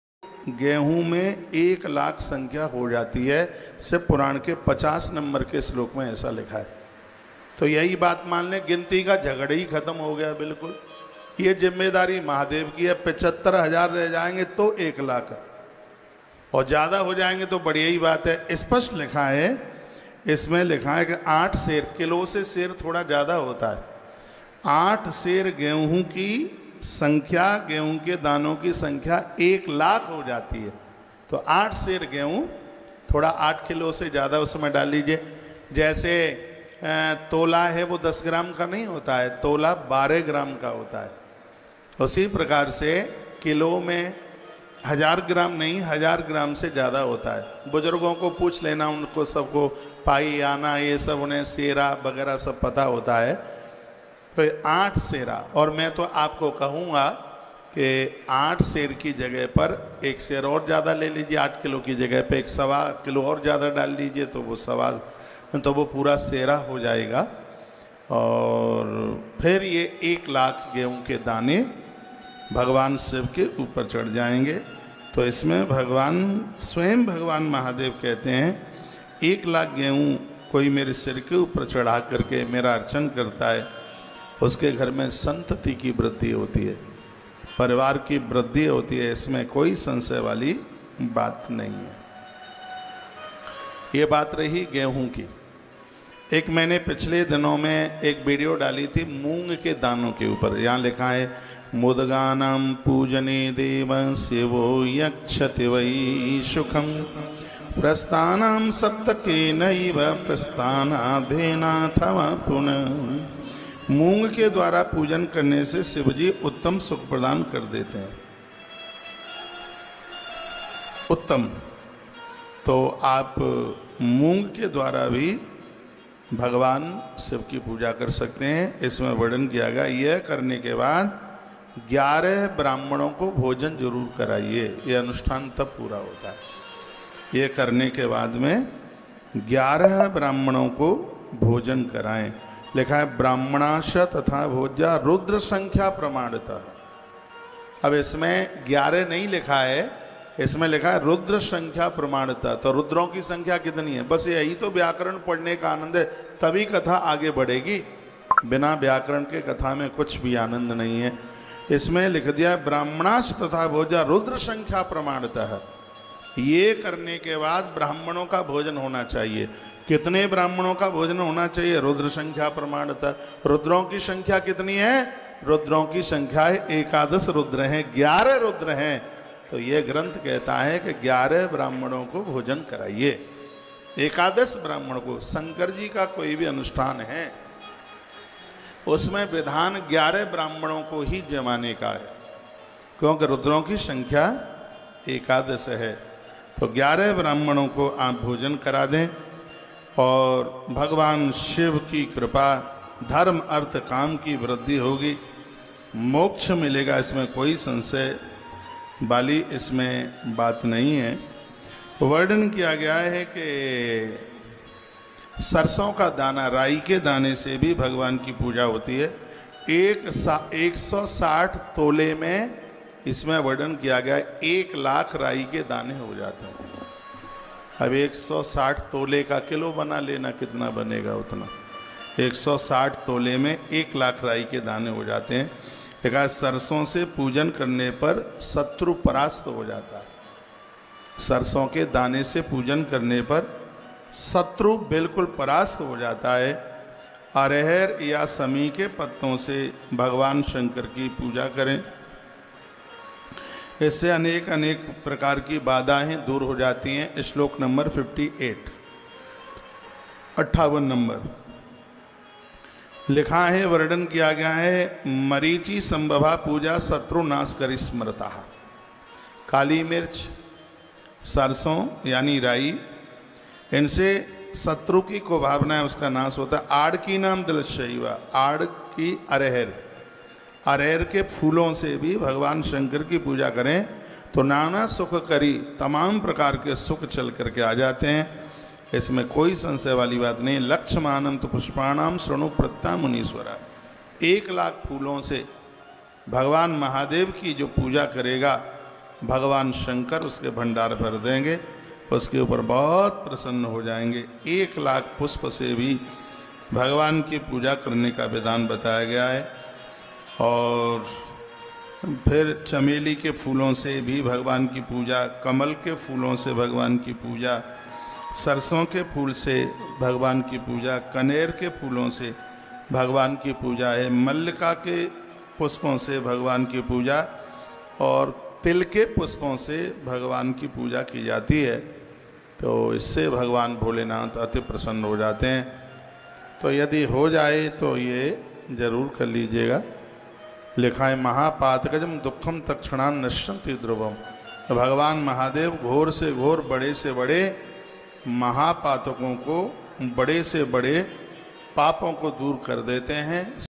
आठ शहर गेहूं में एक लाख दाने होते ही शिव पुराण कथा का कहना हैआगे जाने इसका महत्व#viral
महादेव पर आठ शहर गेहूं चढ़ाने कामहत्व जाना